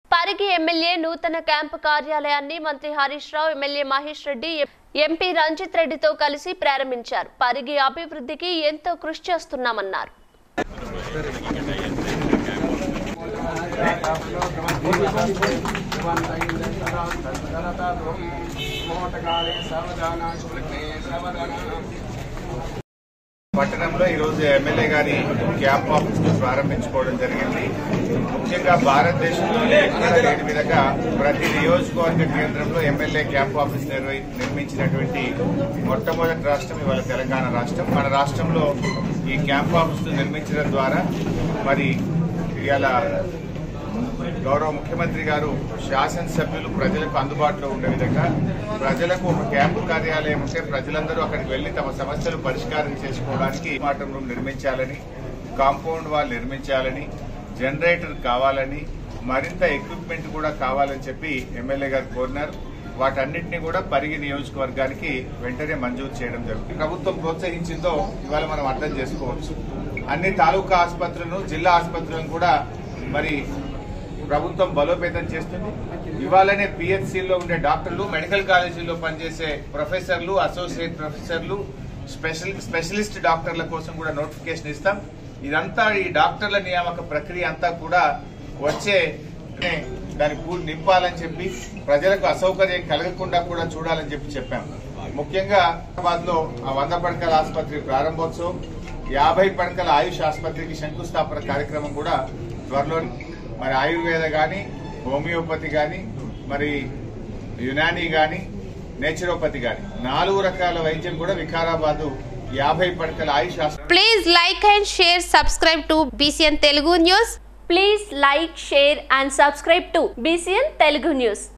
ఎమ్మెల్యే నూతన క్యాంపు కార్యాలయాన్ని ప్రారంభించిన మంత్రి హరిష్ రావు
పరిగి ఎమ్మెల్యే నూతన క్యాంపు కార్యాలయాన్ని మంత్రి హరిష్ రావు,  ఎమ్మెల్యే మహేష్ రెడ్డి, ఎంపి రంజిత్ రెడ్డి tho కలసి ప్రారంబించారు.పరిగి అబివృద్దికి ఎంతో కృషి చేస్తున్నామని తెలిపారు.